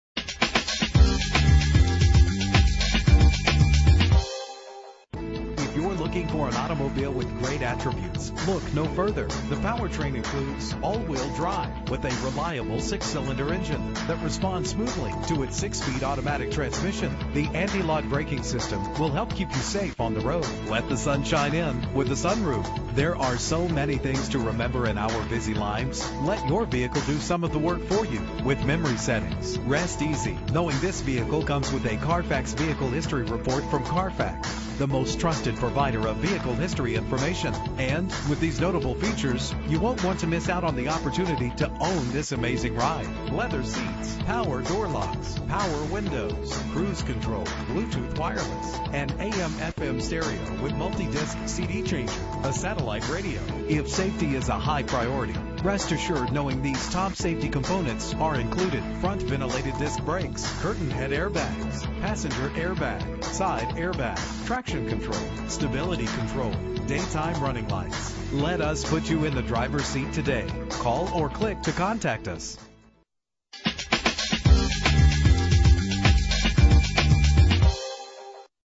2009 Volvo S80 - Williamstown NJ
Year: 2009
Make: Volvo
Model: S80
Trim: T6
Engine: 3.0 liter inline 6 cylinder 24 valve
Transmission: 6-Speed Automatic
Color: Gray
Mileage: 34836
Address: